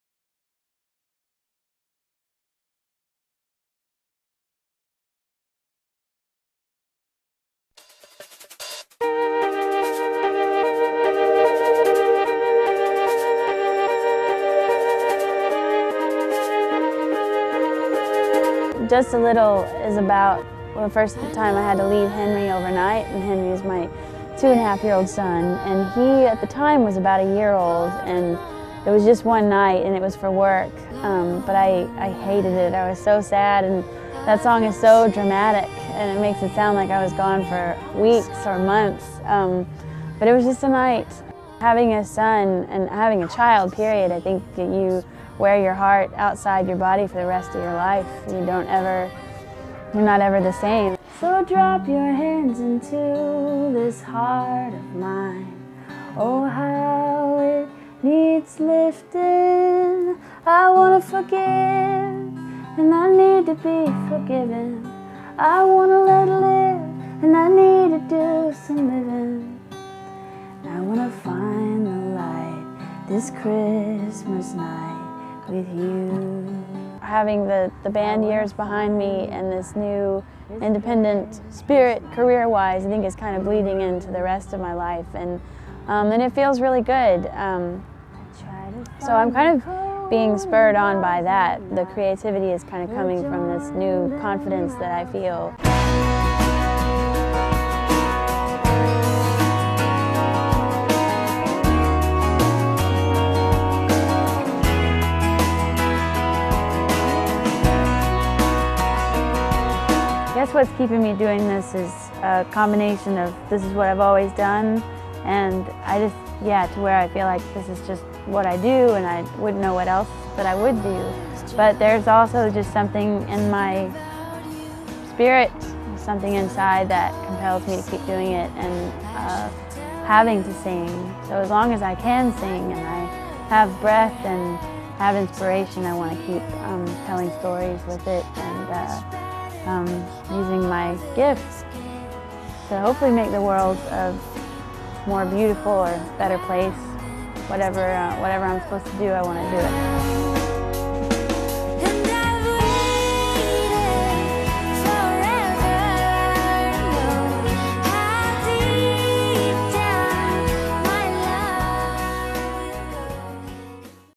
Leigh Nash: Speakerheart
Leigh Nash discusses how having a child changes you forever.